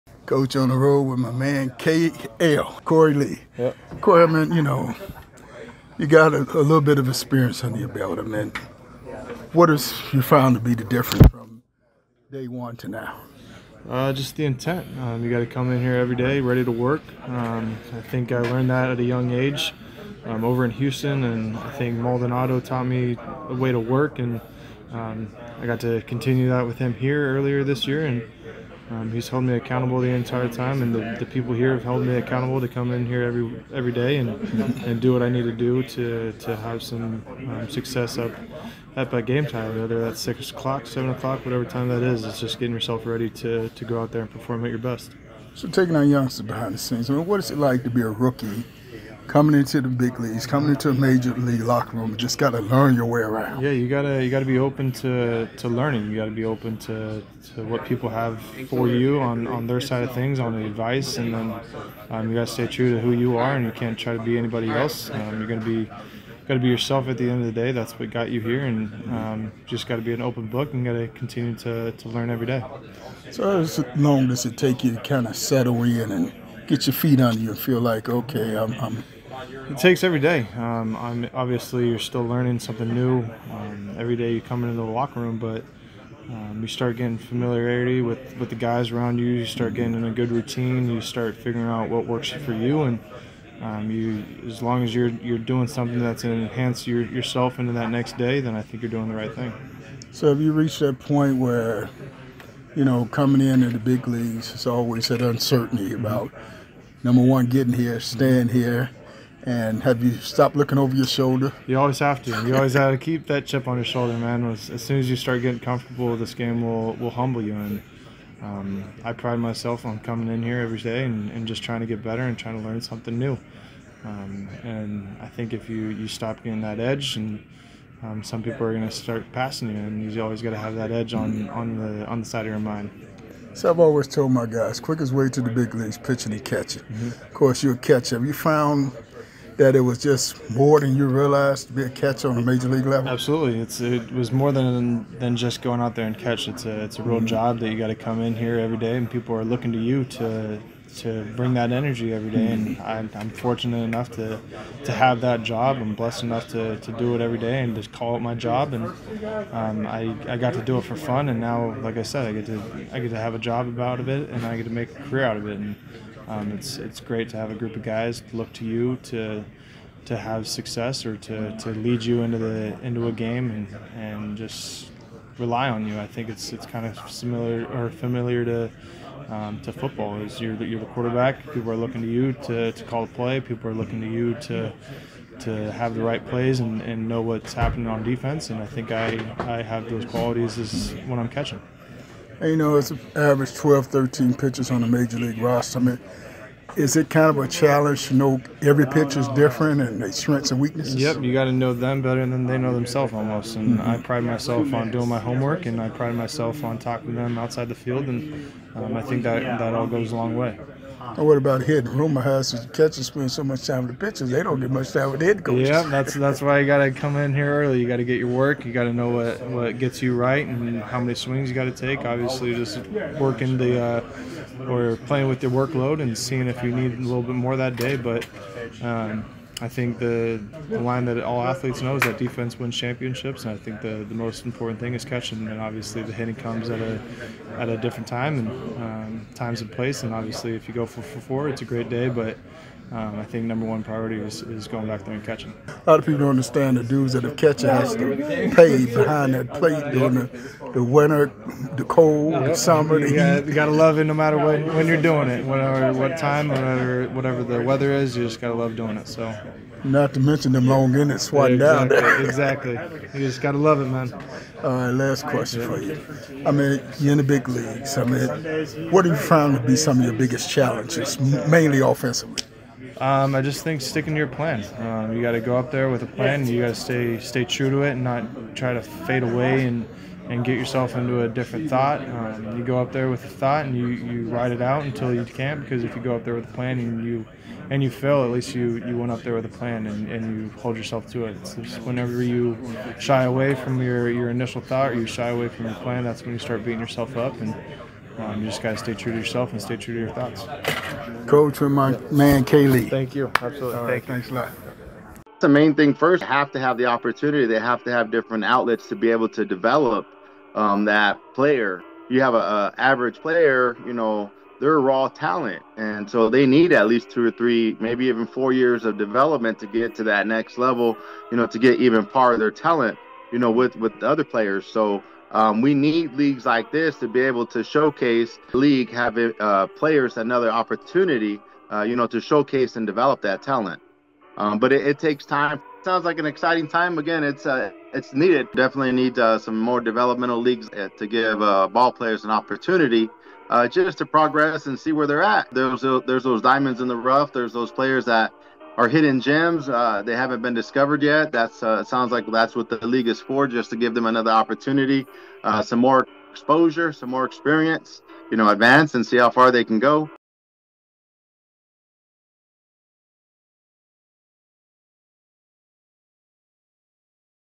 Korey Lee of the White Sox engages in a one-on-one discussion with Coach Michael E.
Coach Mike Mayden conducts comprehensive interviews with former and current MLB stars.